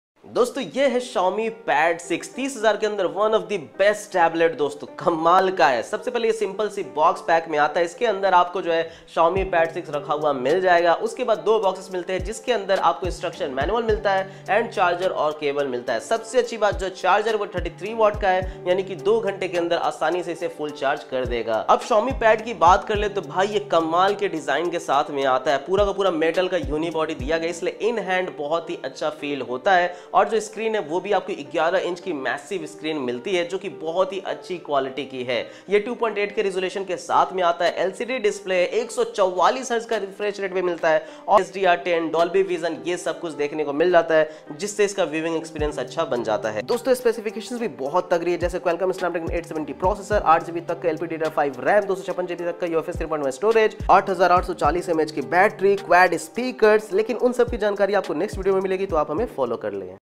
Xiaomi Pad 6 under 30,000/- best Tablet in this price range..
Xiaomi Pad 6 under 30000 Best Tablet with Dolby Vision Atmos and very famous Snapdragon 870, quad Stereo Speakers, 144 Hz Cinematic Display and 8840 mah big battery.